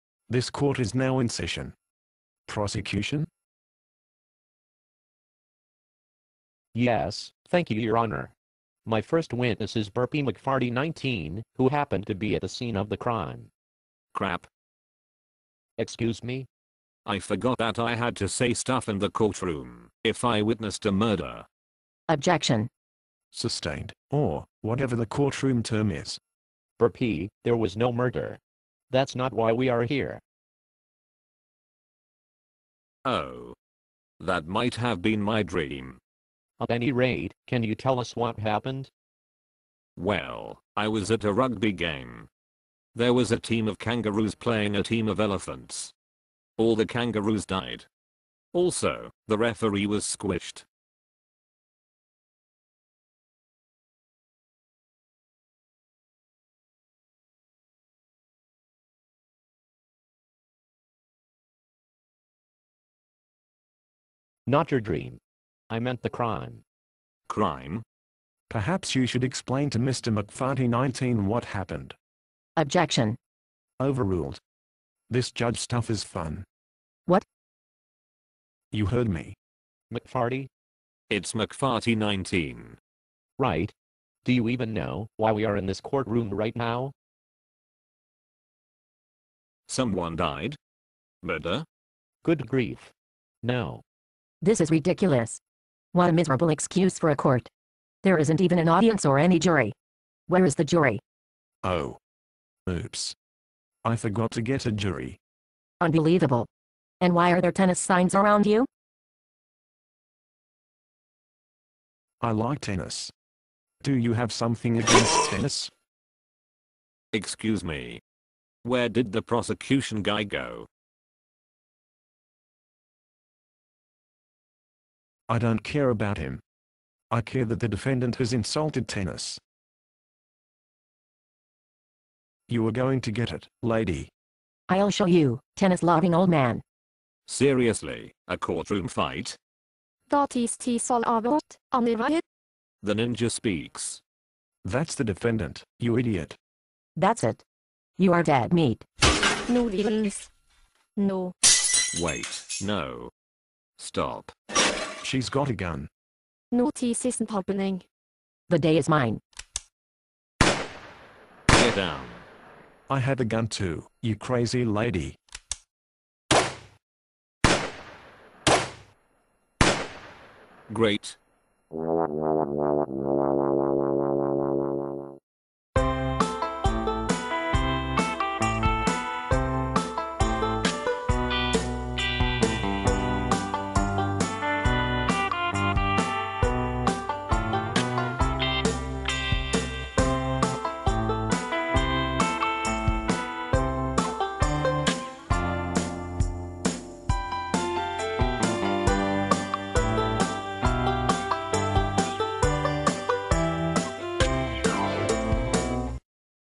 "Courtroom Madness" - A Burpy McFarty 19 Short
Burpy McFarty 19 in a courtroom? That's not good. Madness is sure to ensue!

Rate and comment, if you are so inclined.

(Re-uploaded to fix various issues)